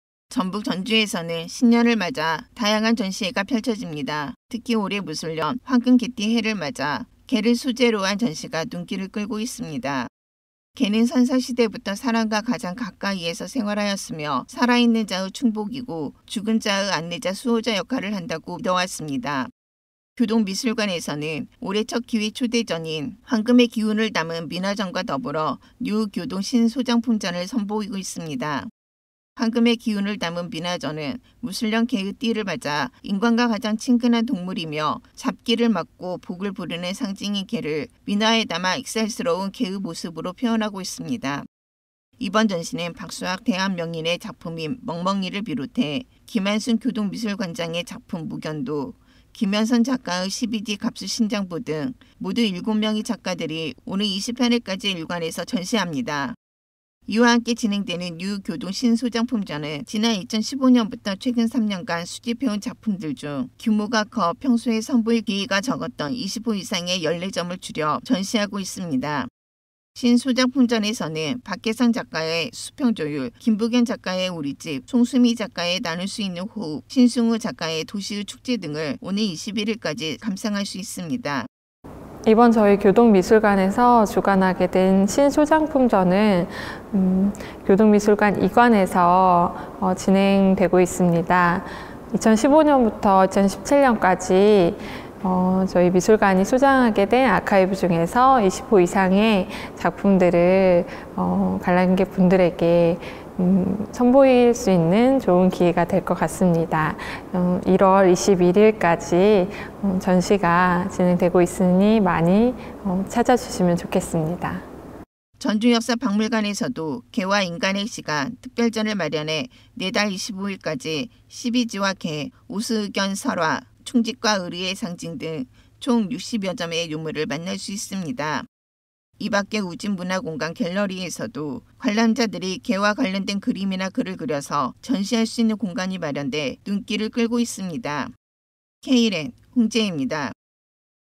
전주, ‘무술년(戊戌年) 황금개’ 담은 전시 ‘눈길’
민화, 유물 등 개 소재 작품 선봬